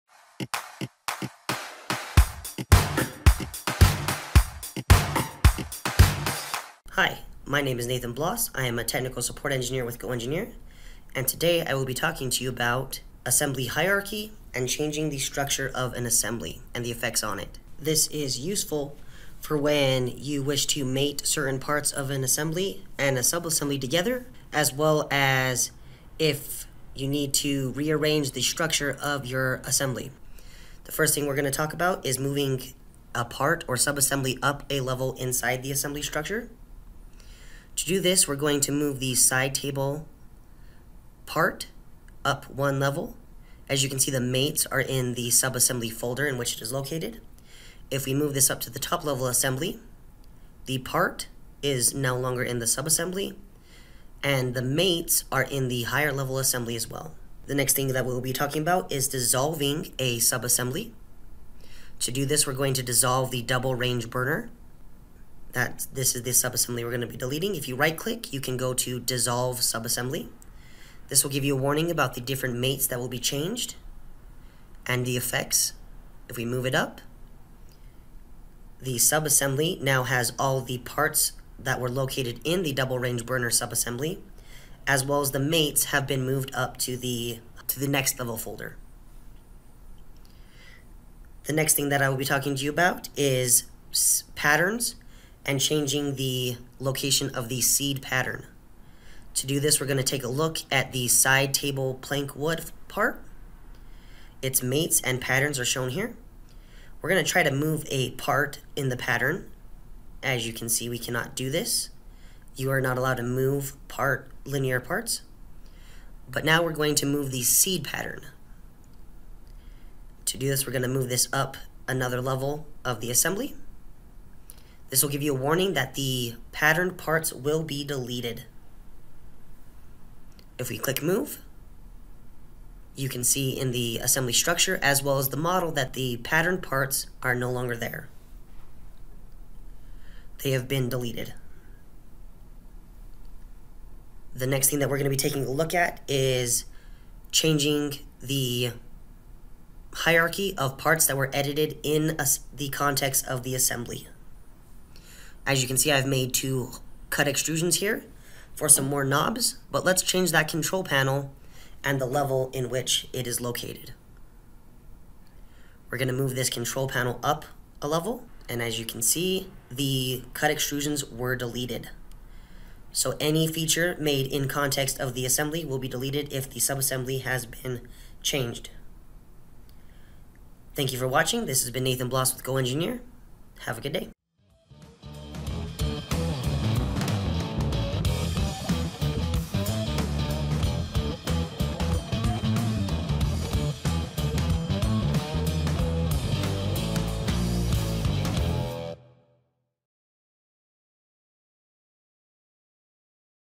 SOLIDWORKS - Assembly Structure Editing
Learn about the different ways to edit assemblies structure and the effect of these edits in SOLIDWORKS.  Quick Tip presented by Nathen Blas of GoEngineer.